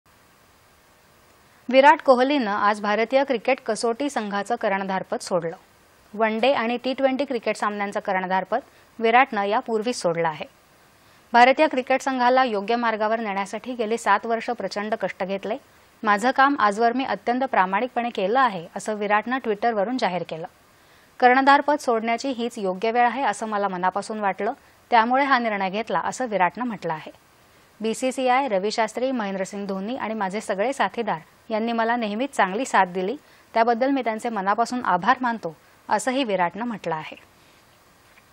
विराट कोहलीनं भारतीय क्रिकेट कसोटी संघाचं कर्णधार पद सोडलं
दि.१५.०१.२०२२ :
  विराट कोहलीनं आज भारतीय क्रिकेट कसोटी संघाचं कर्णधारपद सोडलं. वन डे आणि टी ट्वेंटी क्रिकेट सामन्यांचं कर्णधारपद विराटनं याआधीच सोडलं आहे.
    भारतीय क्रिकेट संघाला योग्य मार्गावर नेण्यासाठी गेली सात वर्ष प्रचंड कष्ट घेतले. माझं काम आजवर मी अत्यंत प्रामाणिकपणे केलं आहे असं विराटनं ट्विटरवरुन जाहीर केलं. कर्णधारपद सोडण्याची हीच योग्य वेळ आहे असं मला मनापासून वाटलं म्हणूनच हा निर्णय घेतला असं विराटनं म्हटलं आहे. बीसीसीआय, रवी शास्त्री, महेंद्रसिंग धोनी आणि माझे सगळे साथीदार यांनी मला नेहमीच चांगली साथ दिली त्याबद्दल मी त्यांचे मनापासून आभार मानतो, असंही विराटनं हया ट्विटर संदेशात म्हटलं आहे.